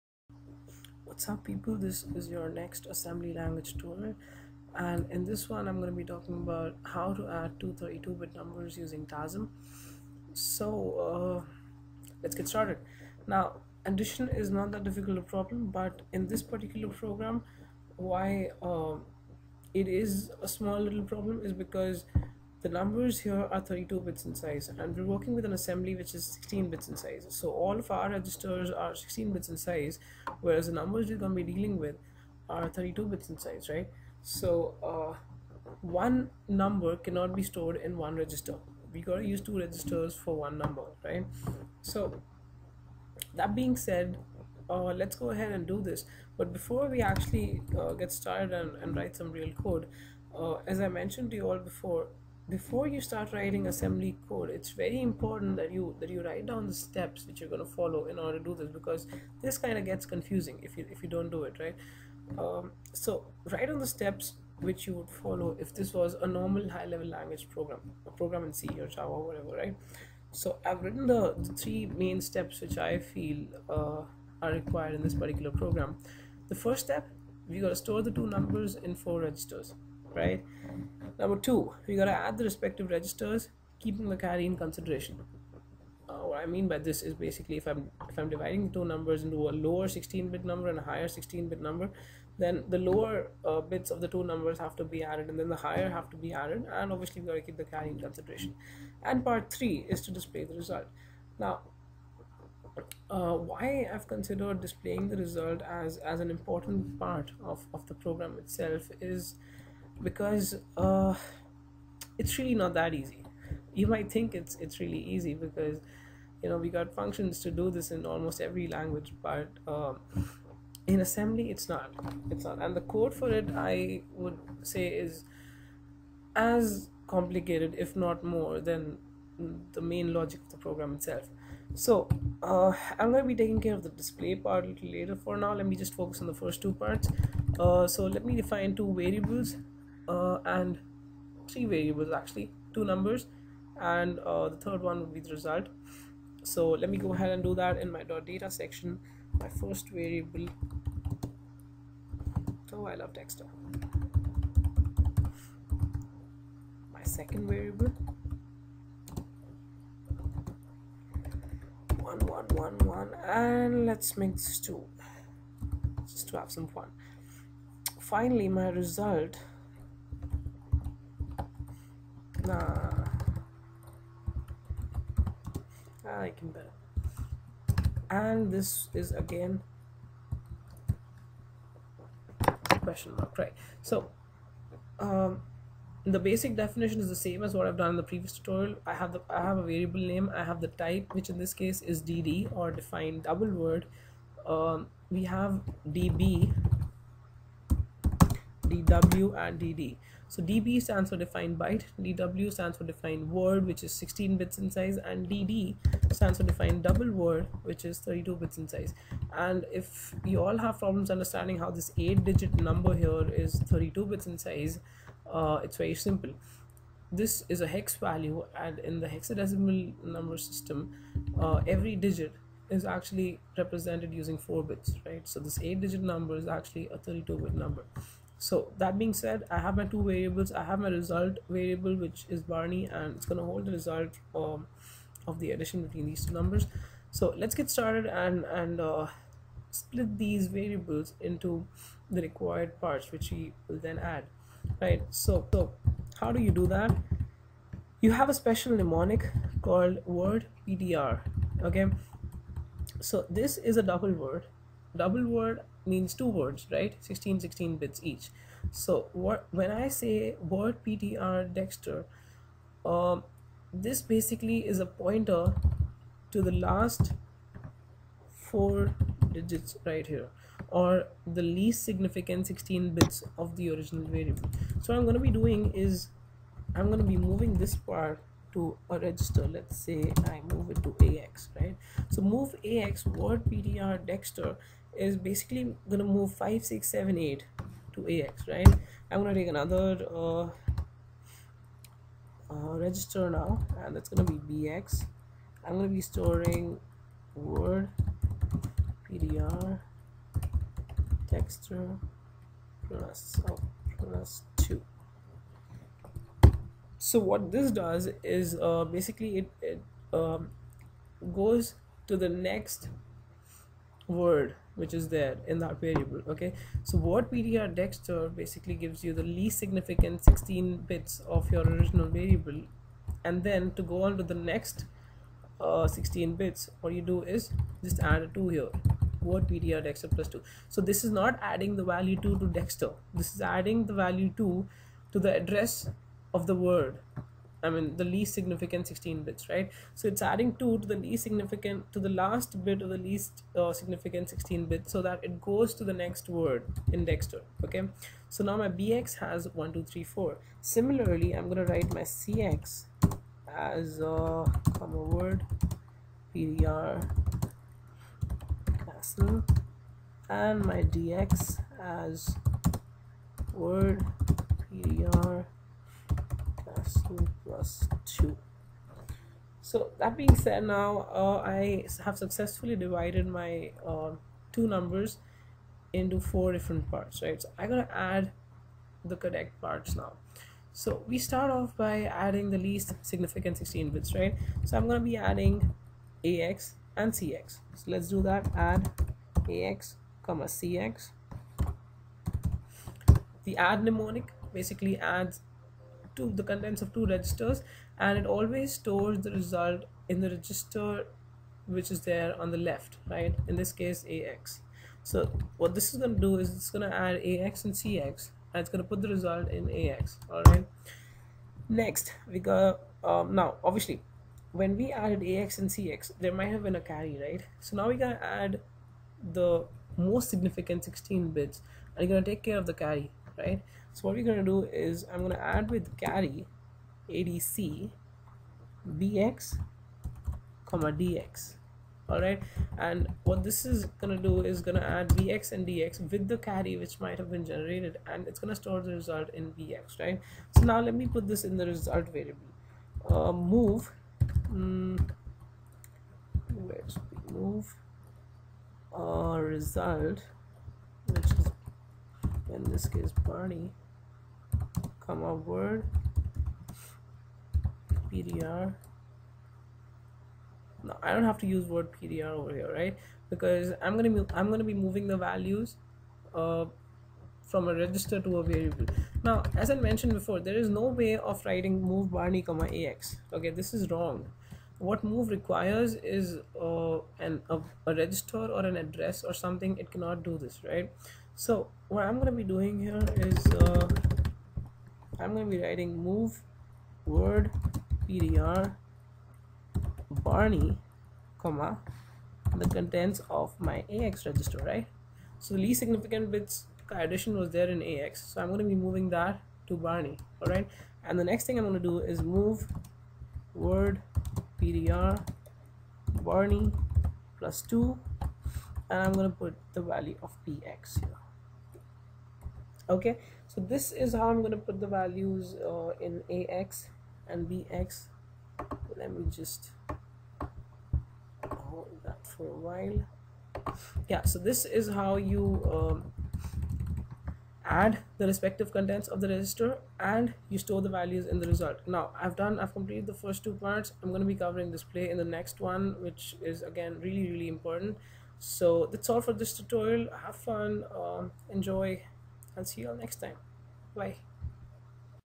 Assembly Tutorial 7 - Add 32+32 = 32
In this tutorial, I talk about how to add two 32 bit numbers in tasm.
Displaying the result of the operation is done in the next tutorial so make sure you watch that as well!!